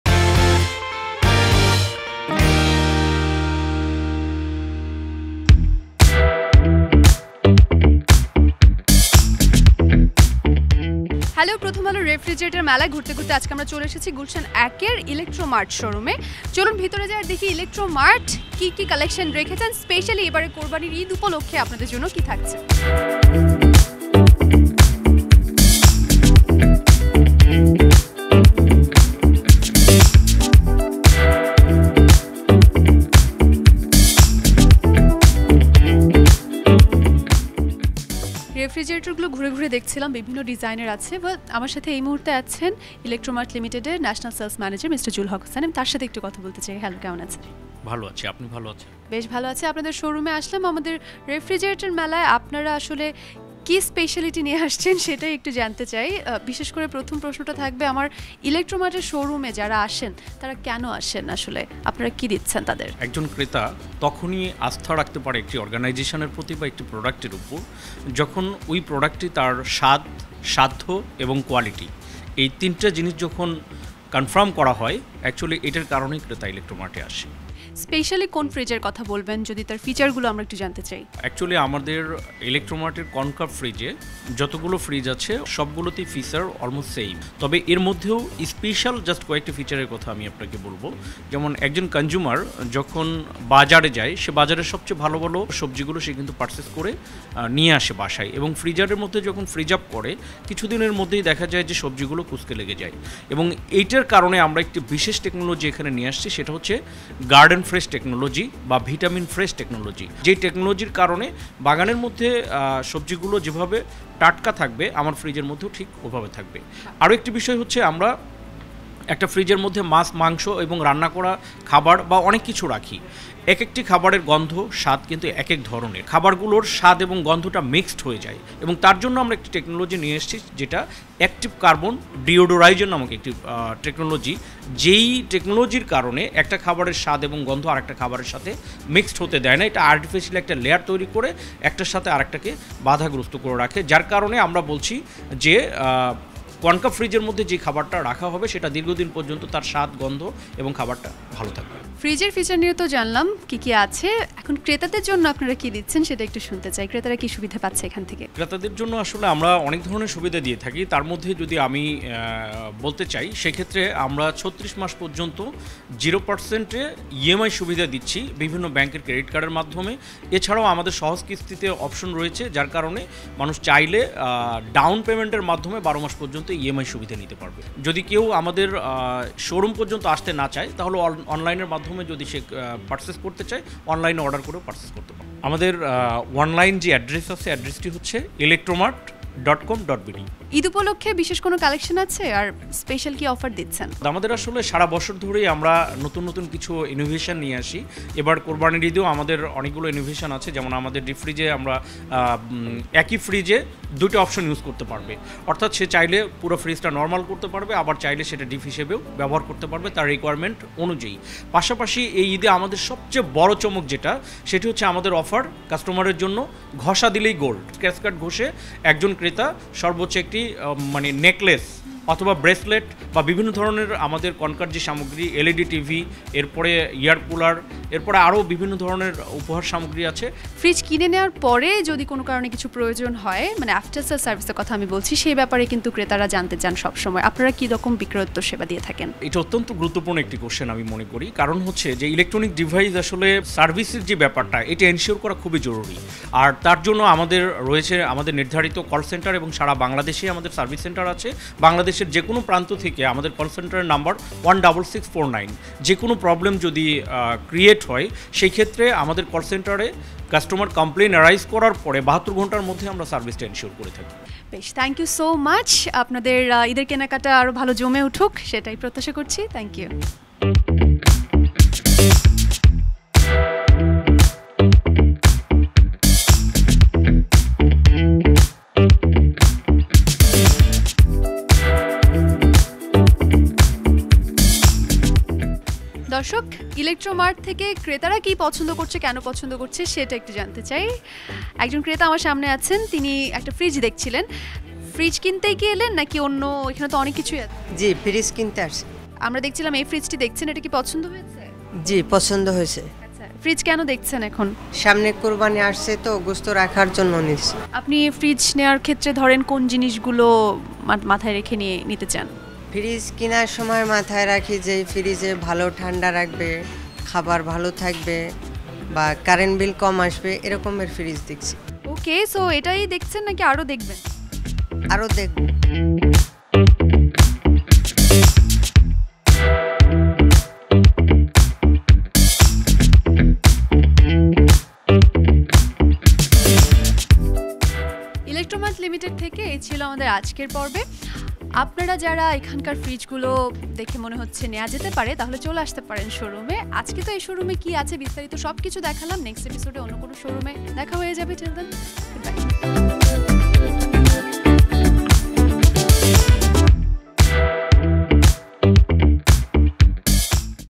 সাধ, সাধ্য আর গুণগত মানের সংমিশ্রণ কনকা রেফ্রিজারেটর | Refrigerator Mela 2024
প্রথম আলো ডটকমের আয়োজনে চলছে 'অনলাইন রেফ্রিজারেটর মেলা ২০২৪'।

মেলায় অংশগ্রহণকারী প্রতিষ্ঠান ‘ইলেক্ট্রোমার্ট লিমিটেডের (কনকা) শোরুম ঘুরে তাদের পণ্যের অফার, নতুনত্ব ও বিশেষত্ব সম্পর্কে জানাচ্ছেন নীল হুরেজাহান।